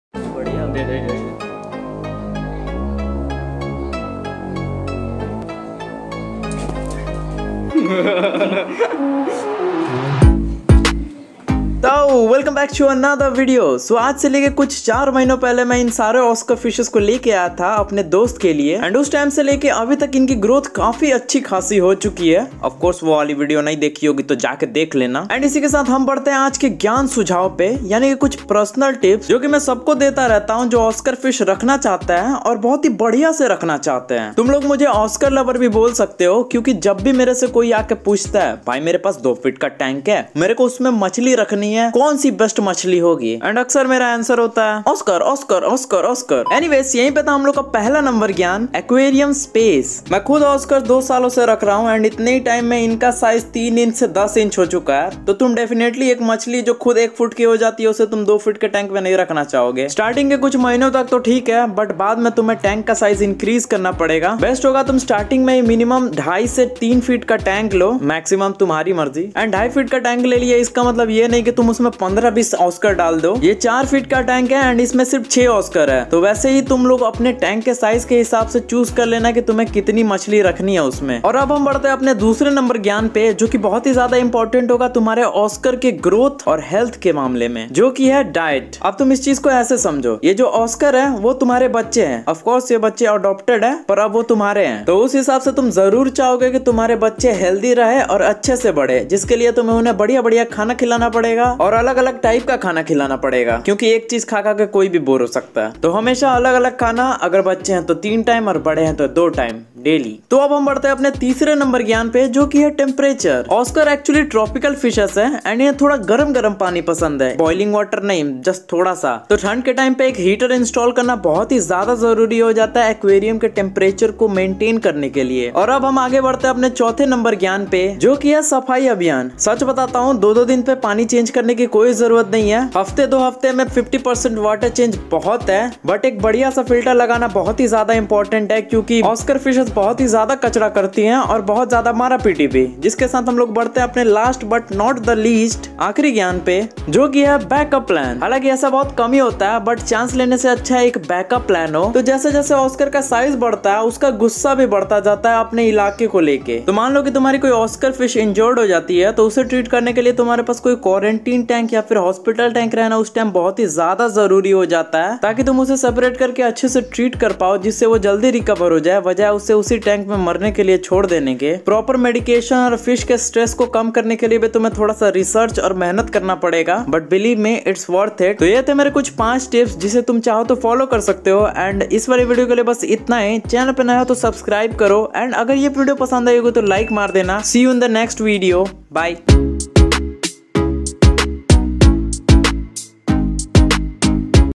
Baby Oscar Fish Growth | Top 5 Tips To Keep Oscar fish - Tiger Oscar & Albino Red Oscar
Oscar fish are so so amazing.
Tiger Oscar and Albino Red Oscar fish.